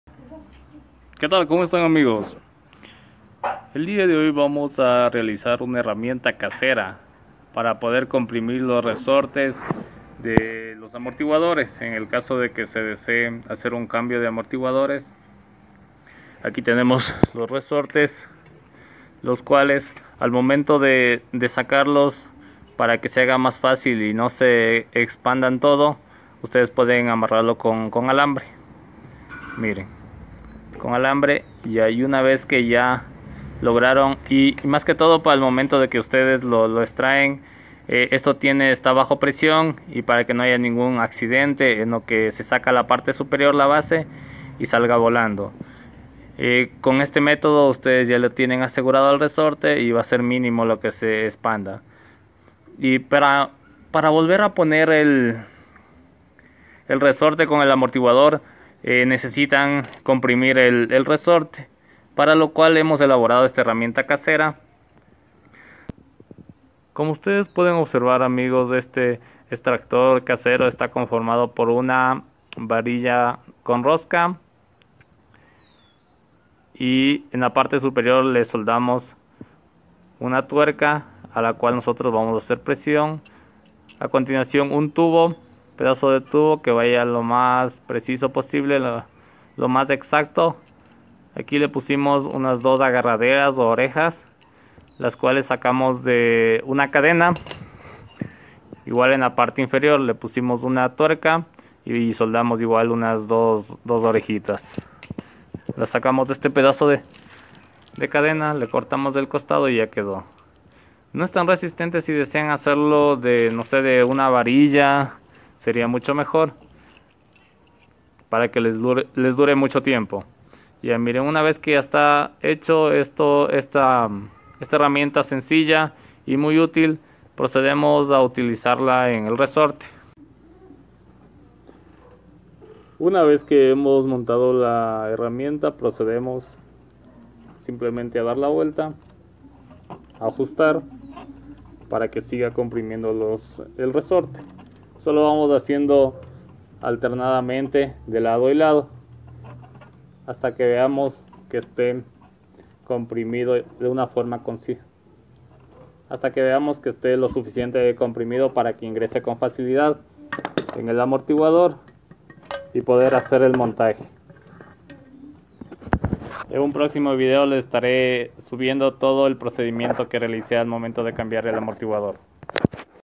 Prensa espiral casera para AMORTIGUADORES
Realizamos una prensa para poder extraer los resoltes del amortiguador de su vehiculo facil y rapido en casa